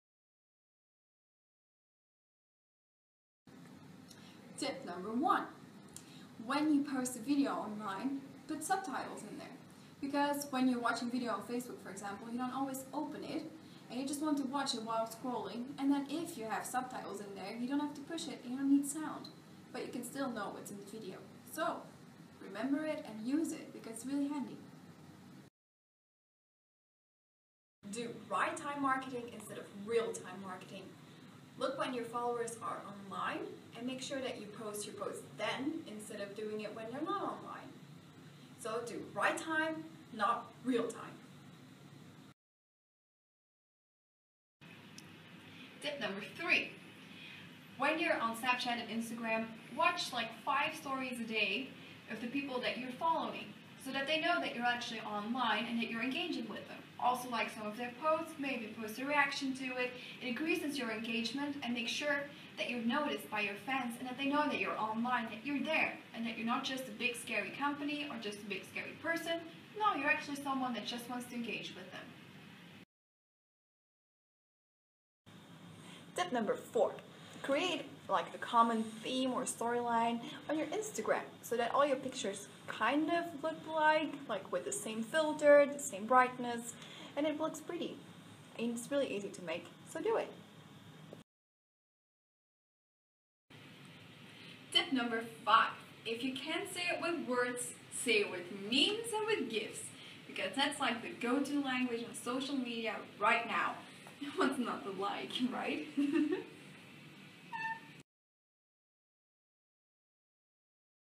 5 Social Media Marketing Tips To Live By - Laura Embrechts
5 social media marketing tips will make reading your content easier and more enjoyable your followers and also increase your popularity - so read on!

Here are more places where you can learn from the Social Media Jedi -